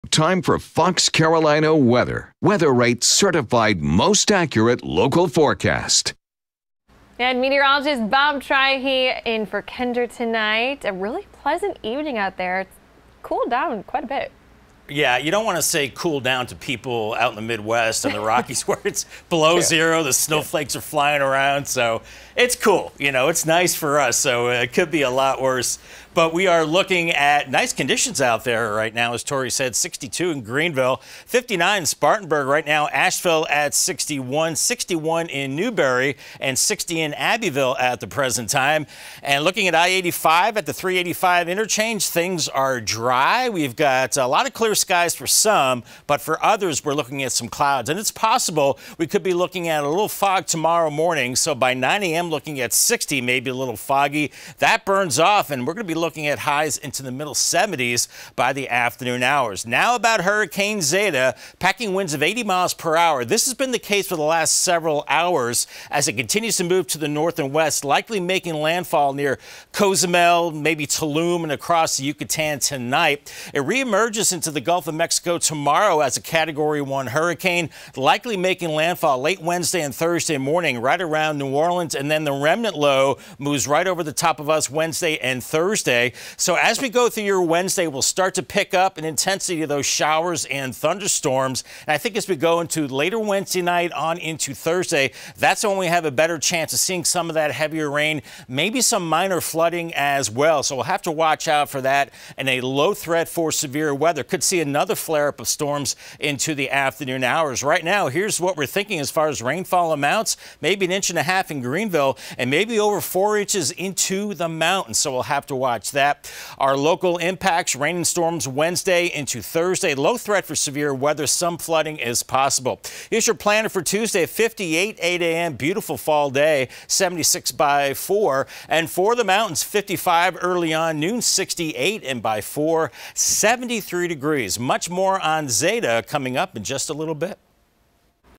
Warm weather to continue through Tuesday
Bob Trihy delivers a full forecast and continues to track the progress of Zeta, which is set to become a Hurricane